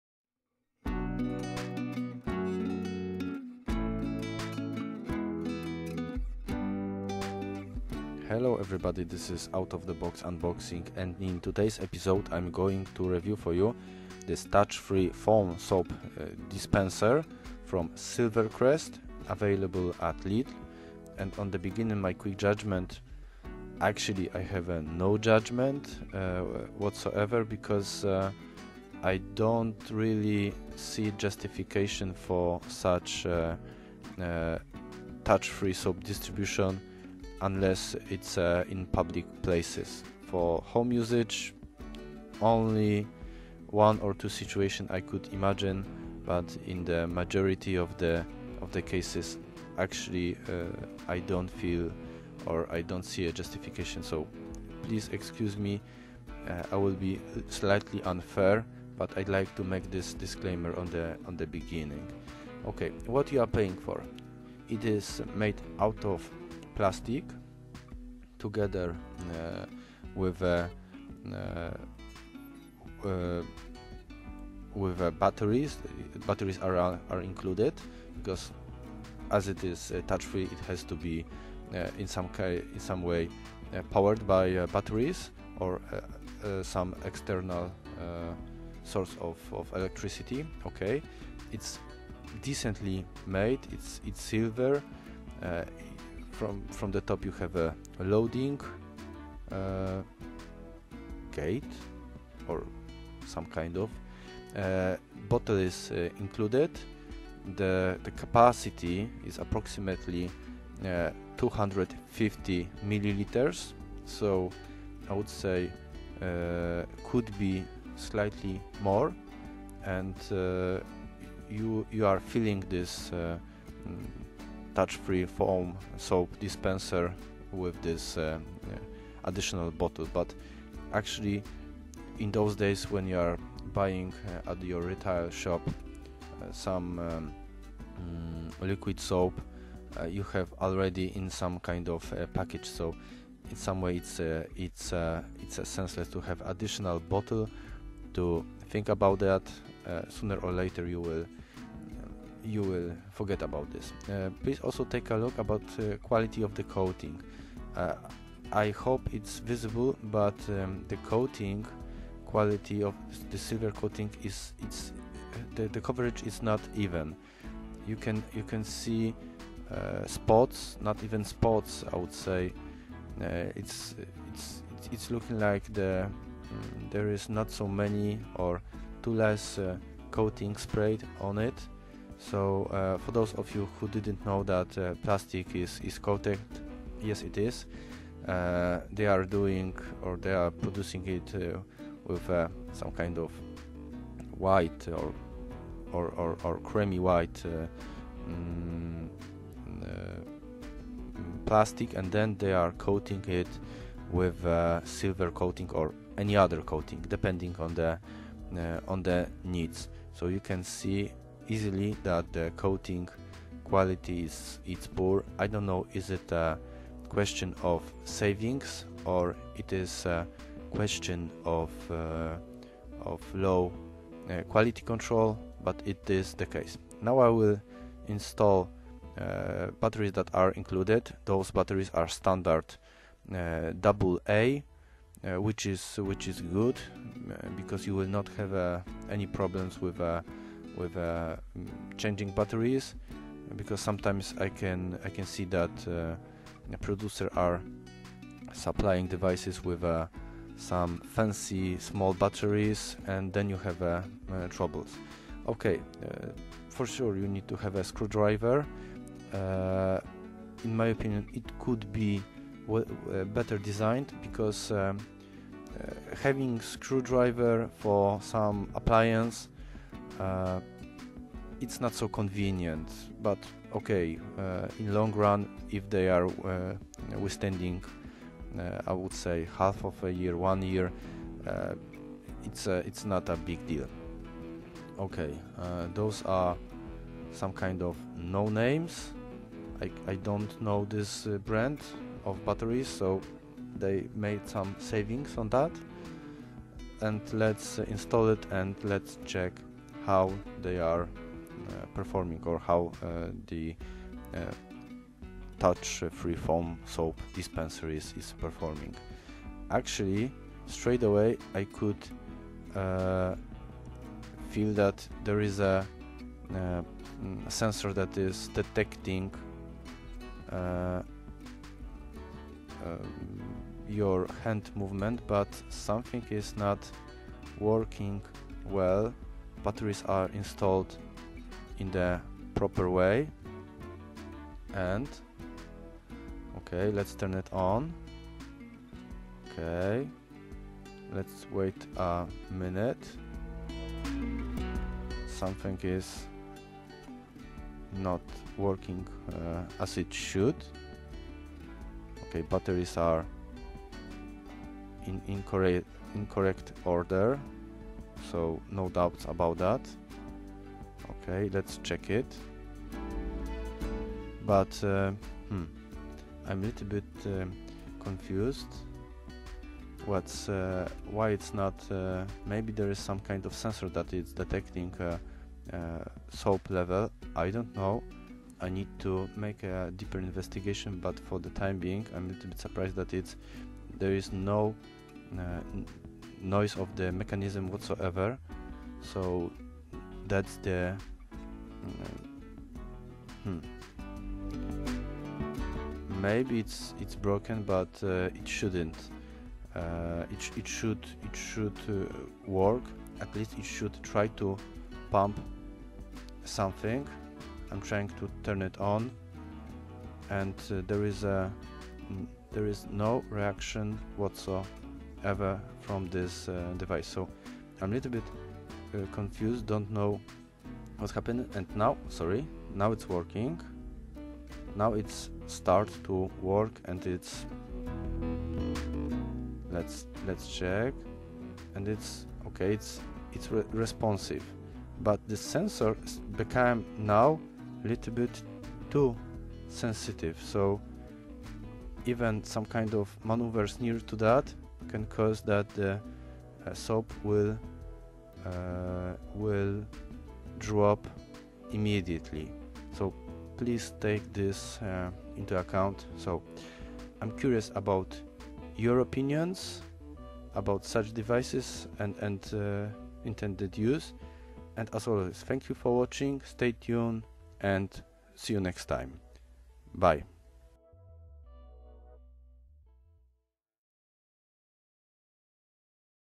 SilverCrest Touch-Free Foam Soap Dispenser SSSES 1.5 A1 REVIEW (Lidl IPX4 250ml)
IAN: 309966

Features
Touch-Free Foam Soap Dispenser
Hygienic - touch-free operation
Add water to turn liquid soap into foam soap
Splashproof housing (IPX4)
Fill-level indicator
Non-slip base
Capacity: approx. 250ml
Batteries included